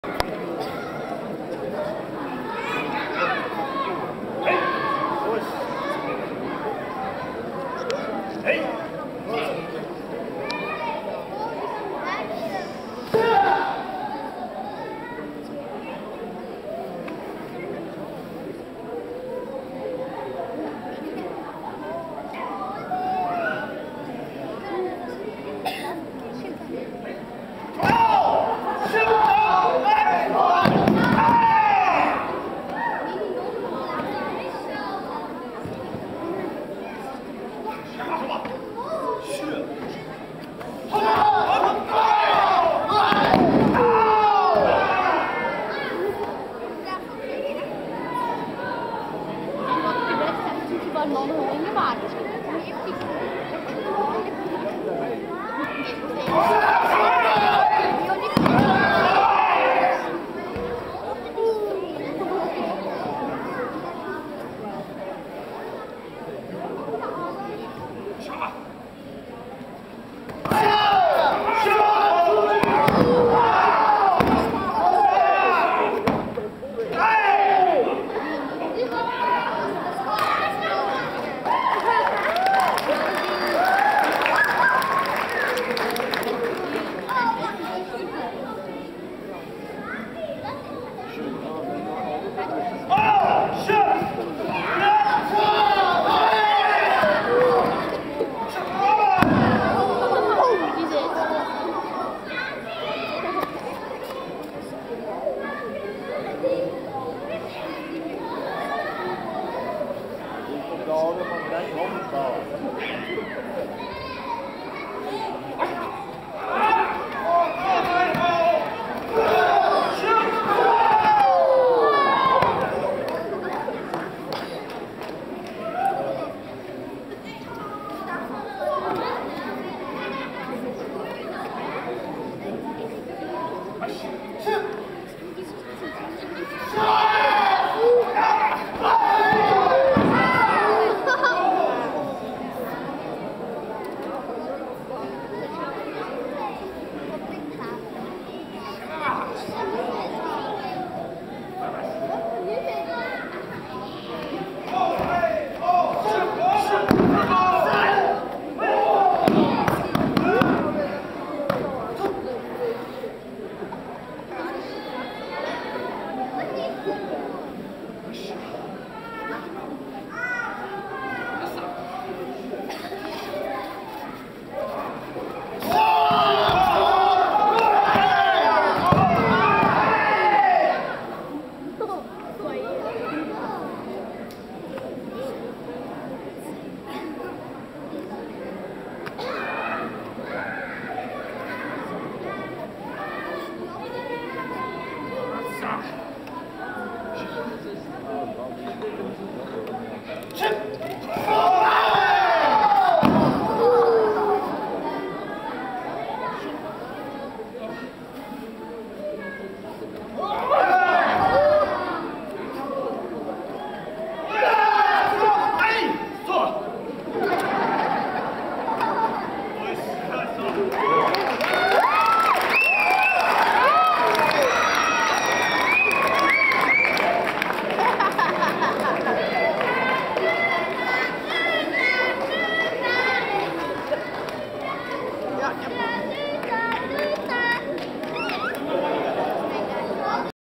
GoNoSen clubkampioenschap 2011 (karate demonstratie)
GoNoSen clubkampioenschap 2011, karate demonstratie uitgevoerd door Eric Bortels, Kevin Megens, Nikki Vandereyt en Roel Ulenaers.